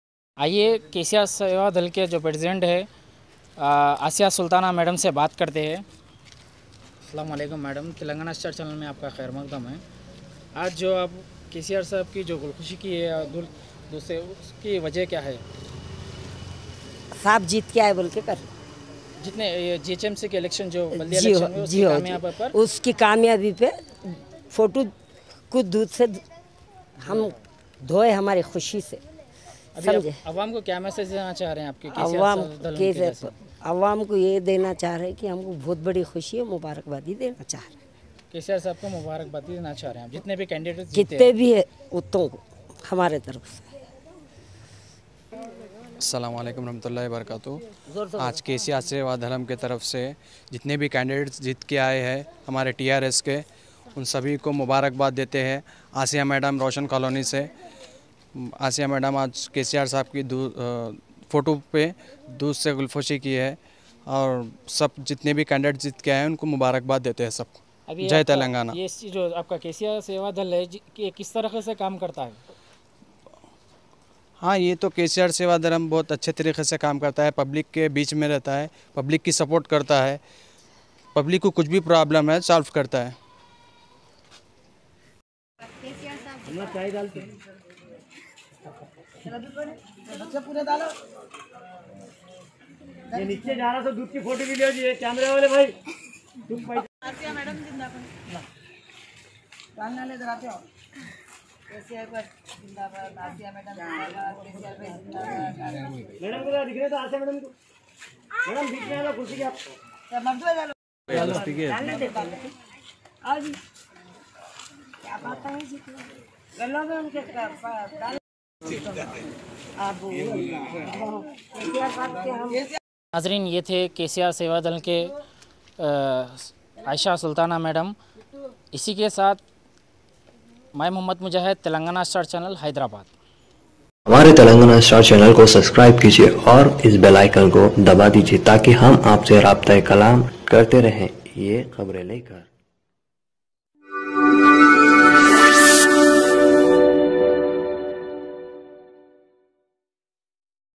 KCR SEVA DALAM KI WOMEN PRESIDENT ASIYA SULTANA MADAM NE ROSHAN COLONY MEIN EK PROGRAM
KCR SEVA DALAM KI WOMEN PRESIDENT ASIYA SULTANA MADAM  NE  ROSHAN COLONY MEIN EK PROGRAM RAKHA  TRS PARTY JITNE KE MAUKE PER